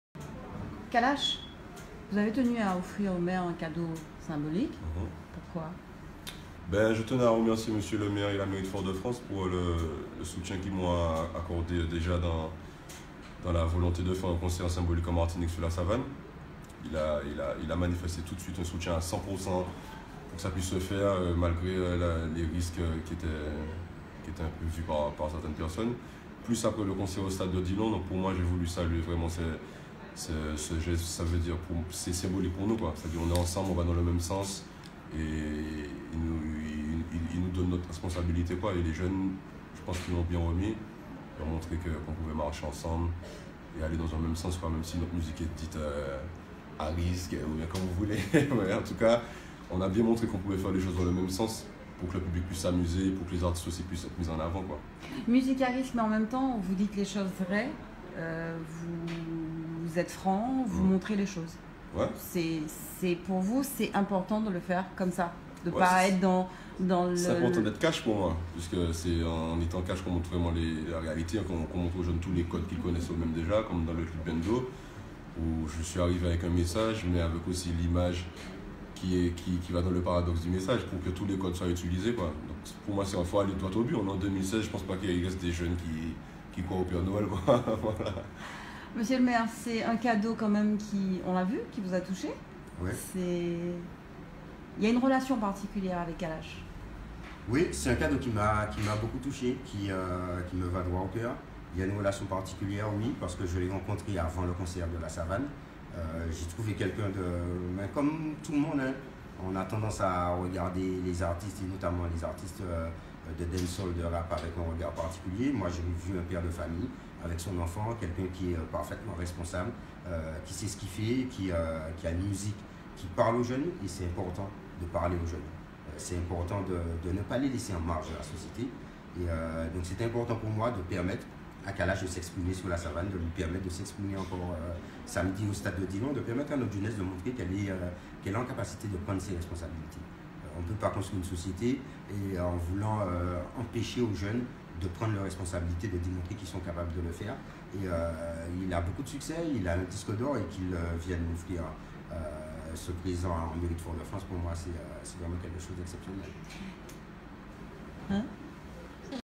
Kalash offre son disque d'or au Maire de Fort-de-France
Cet apres-midi, Kalash a tenu à remercier le Maire, Didier Laguerre, en lui offrant un disque d'or pour son soutien, notamment lors du concert symbolique à la Savane, en juillet dernier et celui de ce samedi 19 novembre, au stade Pierre Aliker. Au cours de cette rencontre, l'artiste a d'ailleurs précisé "Nous allons dans le même sens !"
Un présent qui a touché Didier Laguerre.
Comme l'a rappelé, le Maire " Il est important de montrer que notre jeunesse est capable de prendre ses responsabilités ".

21 novembre 2016 - Ville de Fort de France - Martinique